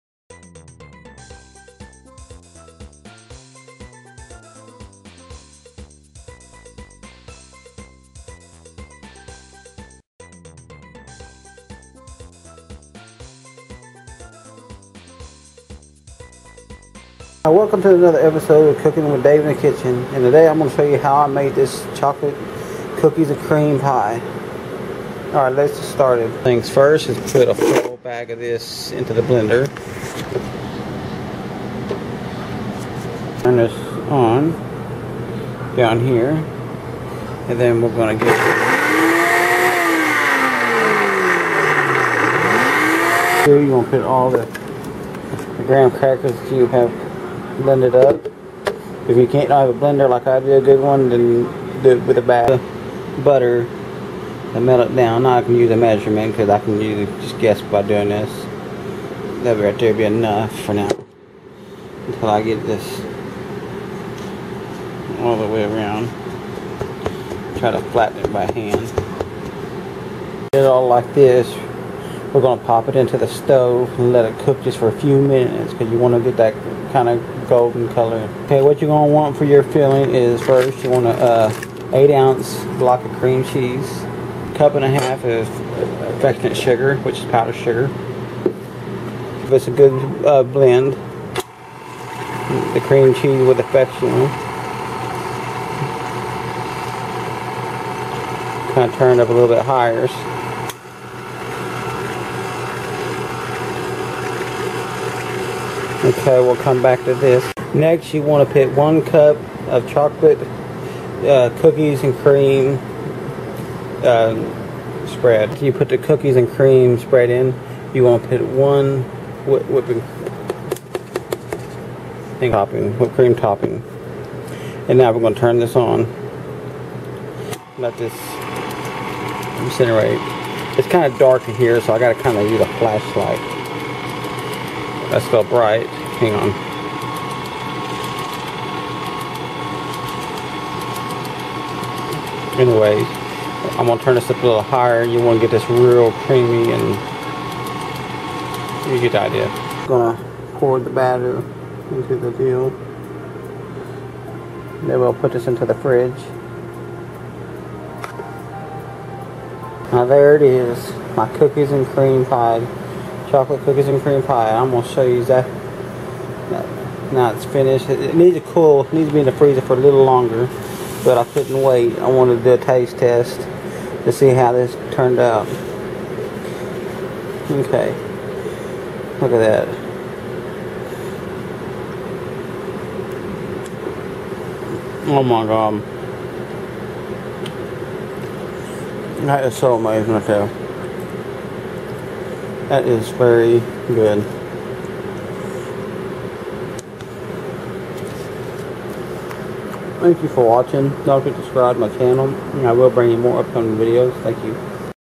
chocolate cookies and cream pie
chocolate cookies and cream pie this pie is so good tasty is so Delicious so watch me make this chocolate cookies and cream pie the recipe for chocolate cookies and cream pie
1 (8 ounce) package cream cheese
1 1/2 cups confectioners' sugar
1 cup chocolate cookies and cream spread
1 cup milk
1 (16 ounce)  frozen whipped topping
 graham cracker crusts